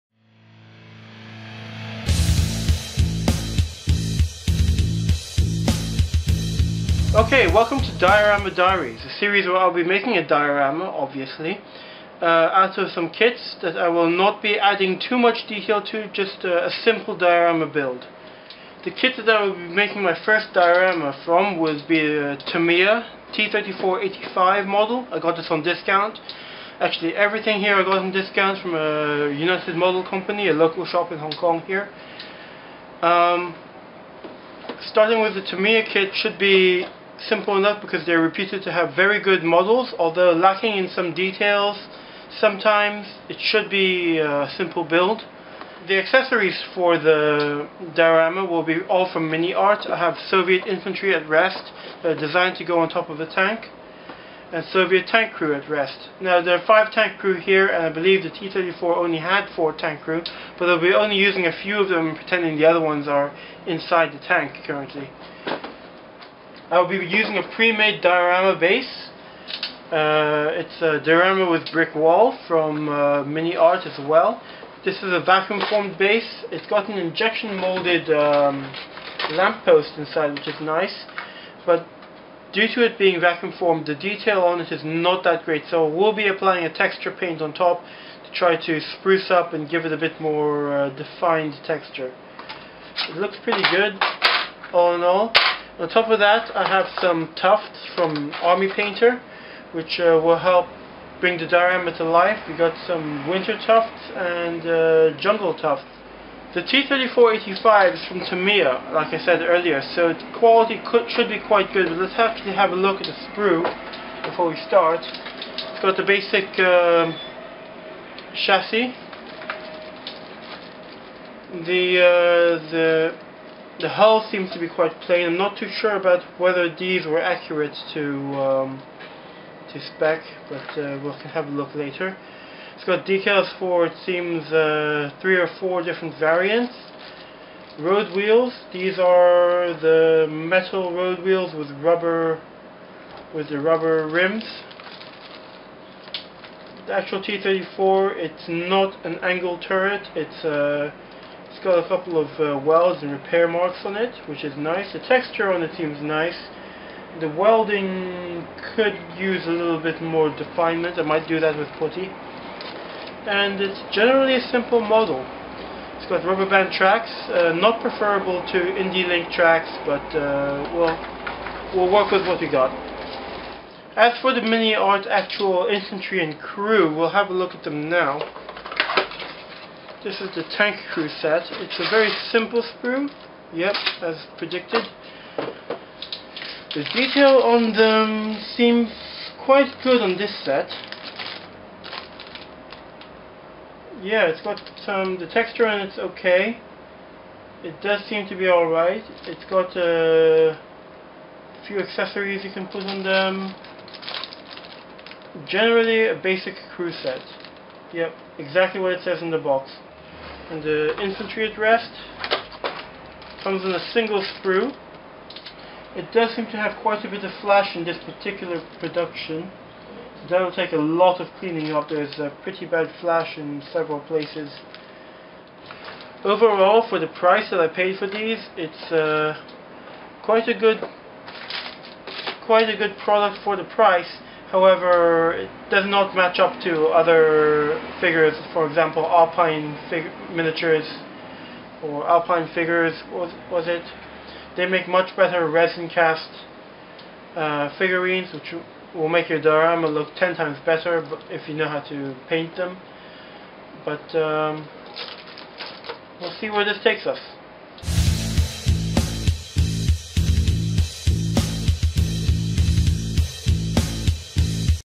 Diorama Diaries T34/85 ep1
In this first installment of Diorama Diaries we go through the kits we will be using to build the project, inspect the quality and gauge the work needed ahead.

Tamiya 1/35 T-34/85
Miniart Soviet Infantry at Rest
Miniart Soviet Tank Crew at Rest
Miniart Diorama with Brick Wall
Army Painter Winter/Jungle Tufts